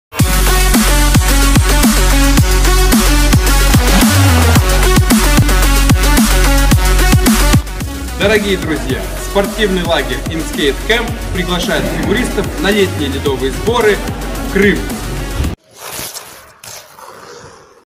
Приглашение на inSkate camp 2021 в Крым
Спортивный лагерь "inSkate camp ЯЛТА 2021" ведёт запись фигуристов на лето!☀️🌴

Ледовые сборы под руководством Алексея Рогонова @Rogonov_Alex
МСМК по фигурному катанию, Олимпийца и создателя мобильного приложения inSkate.

С июня по август в Крыму!🌄
6 смен (5+1+5)!

Выбирайте удобные для Вас даты:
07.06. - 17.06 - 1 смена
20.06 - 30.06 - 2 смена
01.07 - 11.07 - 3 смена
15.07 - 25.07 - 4 смена (Заполнено)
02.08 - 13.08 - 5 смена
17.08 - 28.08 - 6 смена

Места ещё ЕСТЬ!💯

Проживание в комфортабельном ЖК "Паруса" в шаговой доступности от катка!
(оплачивается отдельно)

В программе сборов:
2ч льда;
2ч ОФП+СФП;
1ч Хореография.

2 тренера на льду!👨‍🏫👩‍🏫

Возможны дополнительные тренировки, постановка и отработка программ, работа на "удочке", Power Twist.

Бесплатный звонок:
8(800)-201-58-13

P.S. Рассматриваем сотрудничество с тренерамиℹ